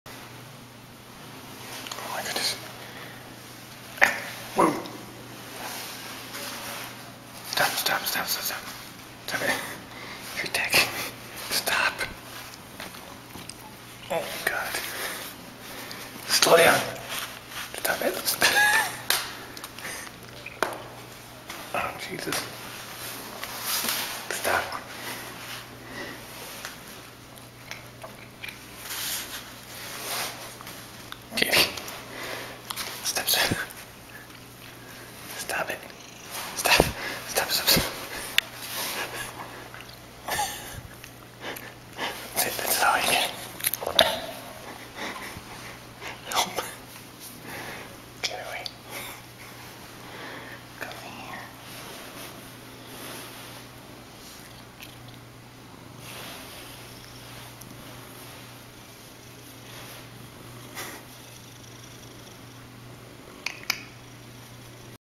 Clicky LOVES Pounce
Stray kitty Clicky attacking me for some Pounce treats.